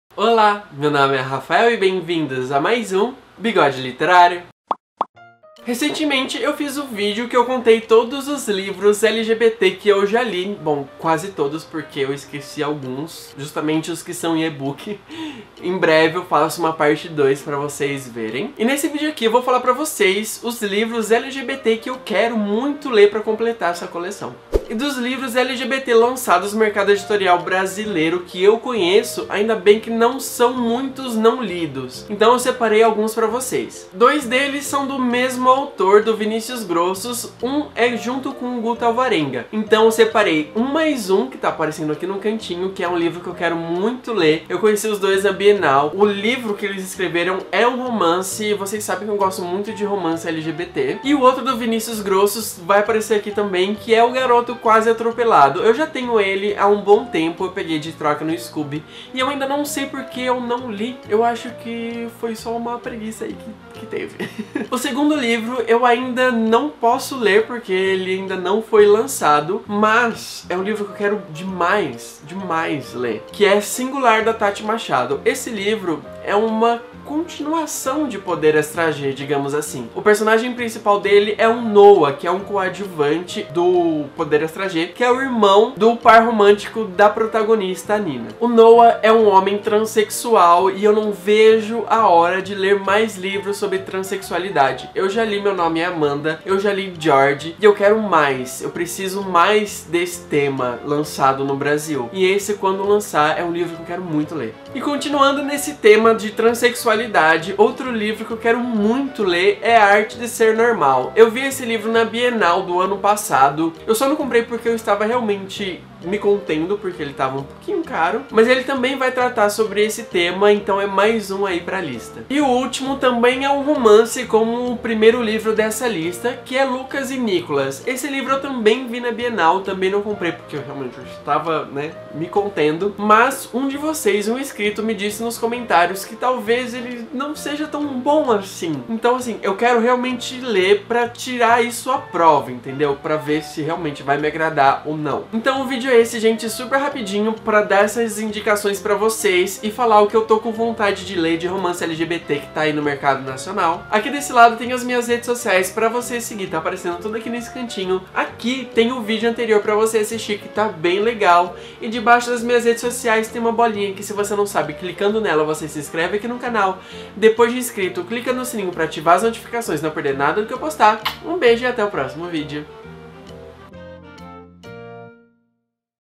LIVROS LGBT QUE EU QUERO LER | BigodeTodoDia | Bigode Literário
Oi gente! Eu já mostrei em um vídeo os livros LGBT que eu já li, nesse eu mostro os que ainda faltam ler. Tem muito romance, livros com personagens transexuais e até romance LGBT nacional.

Livros citados e onde comprar:
1 + 1. A Matemática do Amor
O Garoto Quase Atropelado
A Arte de Ser Normal
Lucas e Nicolas

📚 Minha Wishlist da Amazon

📫 Quer me mandar algo? Olha aí minha caixa postal:

Rafael Ribeiro
Caixa Postal 1513
Bauru - SP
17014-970